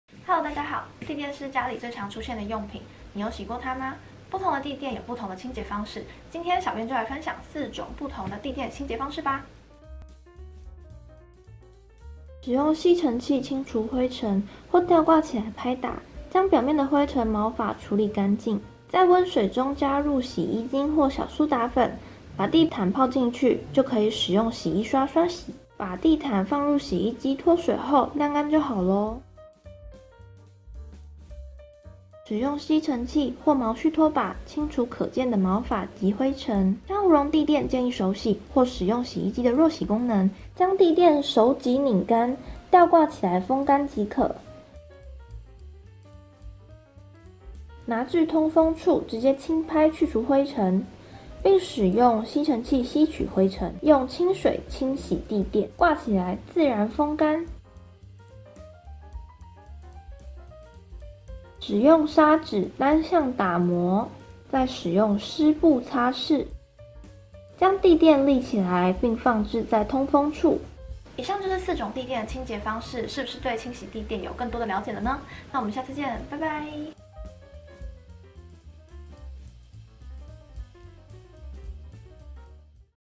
【4種地墊清潔懶人包】你都踩在細菌上嗎?清潔地墊很重要｜Caldo卡朵生活
你平常有清潔地墊的習慣嗎?
地墊很容易累積髒污及細菌,所以一定要記得定期清潔
但是地墊不是靠洗衣機就能洗乾淨的
也是有需要注意的細節及撇步要注意✨

☀ 大家都在追的IG小知識